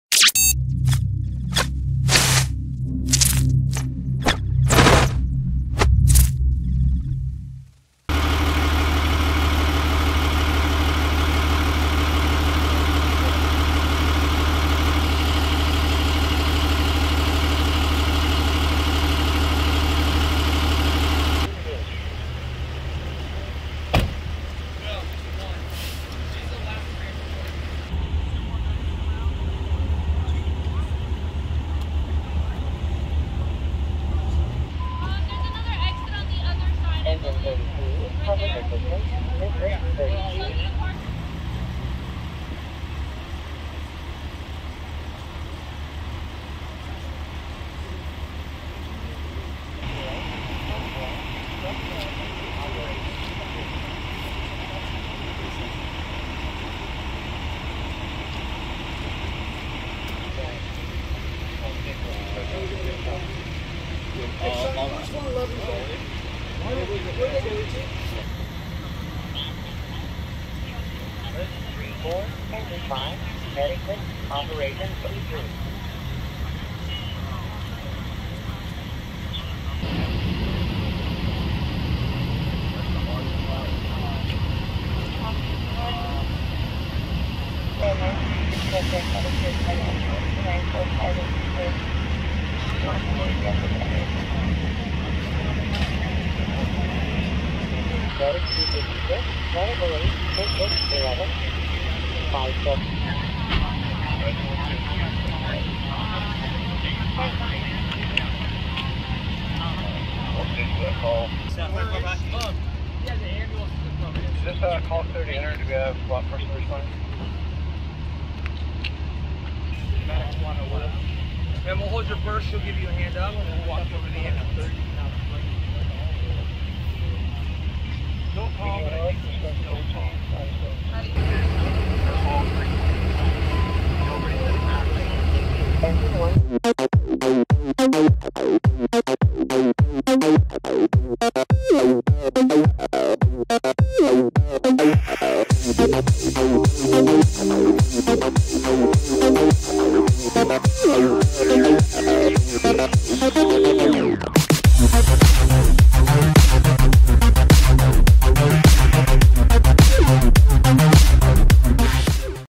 Nine People Injured in Major Accident in Natomas
A major vehicle accident has taken place along Advantage Way in the Natomas area of Sacramento.

The collision took place around 9:45 p.m., and Sacramento Police and Fire responded.

There were nine patients declared on scene, unknown how many of those were taken to the hospital.

A multi-casualty incident was declared due to the number of patients.

Note, this does not mean that anyone has been killed in this crash.